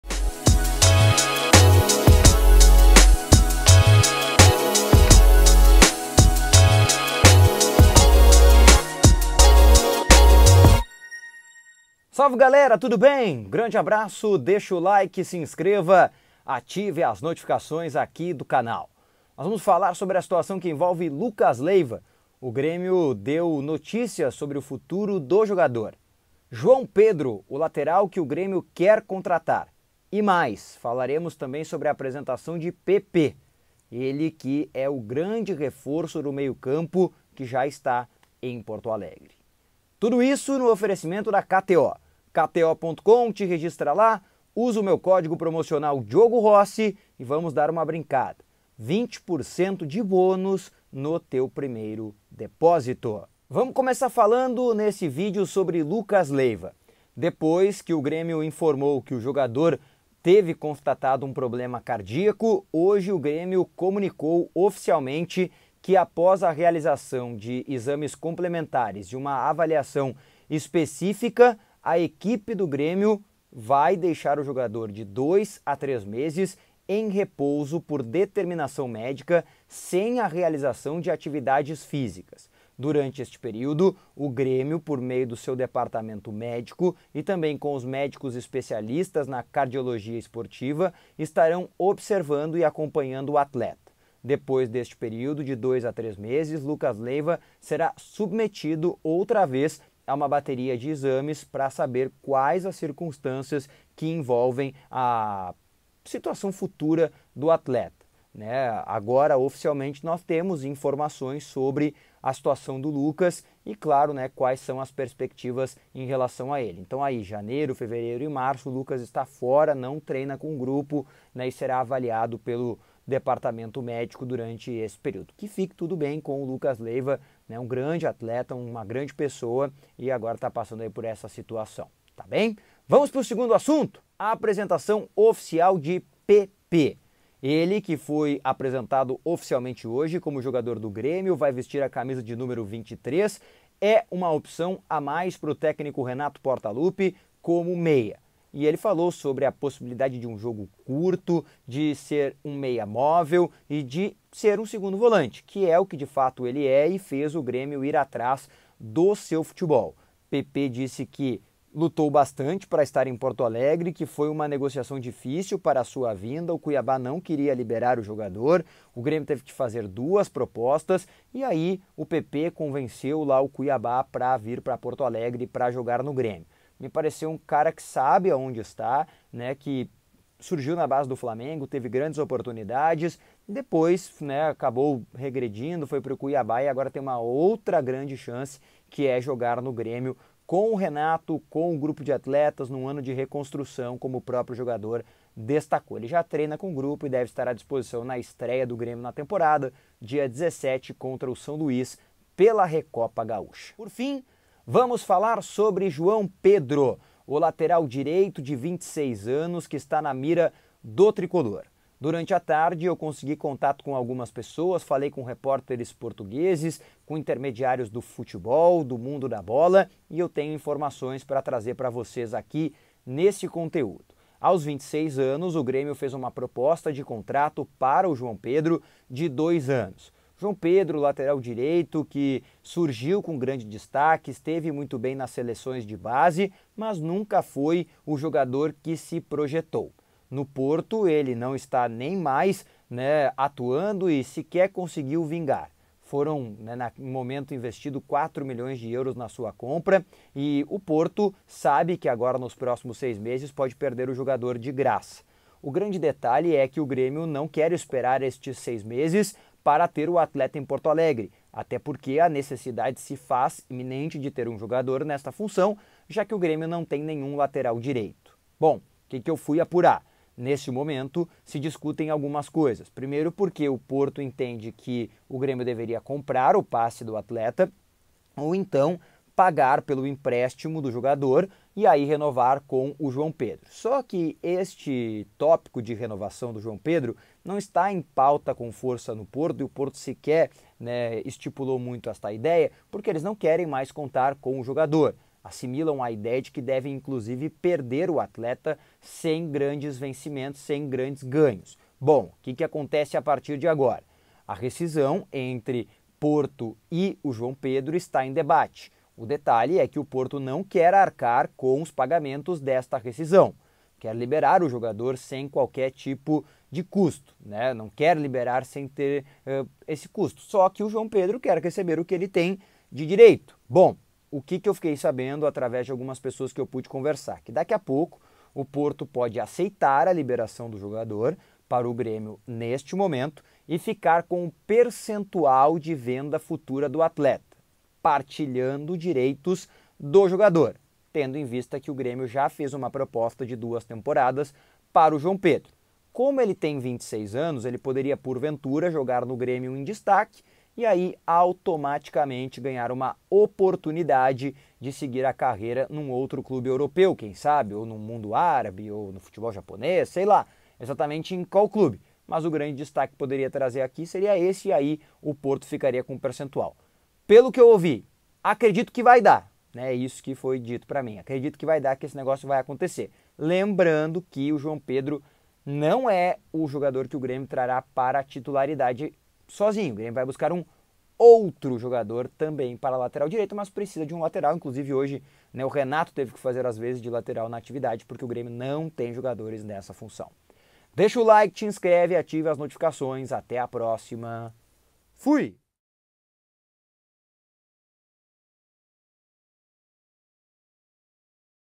DETALHES DO NEGÓCIO QUE PODE ACONTECER | LUCAS LEIVA | PEPÊ E MAIS…
Vou te contar detalhes de um negócio que tá rolando.

Vem comigo!

Oferecimento KTO!